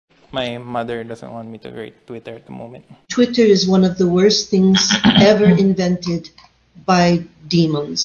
Twitter is 1 of the worst things ever invented by demons. - 9LX WC Wesley So's mom Lotis Key
Related to :
Wesley So is in a secret relationship with Carissa Yip. This is why Wesley has been underperforming (at least in chess) between the 2022 Olympiad and the 2022 Chessc*m Global Championship. / Life imitates art. Theory: The Cleo and Beth thing happened to Wesley So IRL this year.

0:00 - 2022
2022 FIDE Grand Prix - World Champion Wesley So told Dina Belenkaya that Wesley So's mom Lotis Key doesn't want Wesley So to have twitter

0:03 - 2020
2020Aug09 Lotis Key & Wesley So interview by Surya Sekhar Ganguly - Lotis Key says 'Twitter is 1 of the worst things ever invented by demons.'

Context:
They're very religious.

Original 1:
I AM NOT AFRAID OF HIKARU! | Wesley So before the FIDE Grand Prix 2022 in Berlin FINALS
3:28

by:
World Chess

Part of original description:

'GM Wesley So evaluates his chances against Hikaru Nakamura after having qualified for the finals.'

Original 2:
30:35

by:
Surya Sekhar Ganguly

'Join us for an exclusive interview with Wesley So.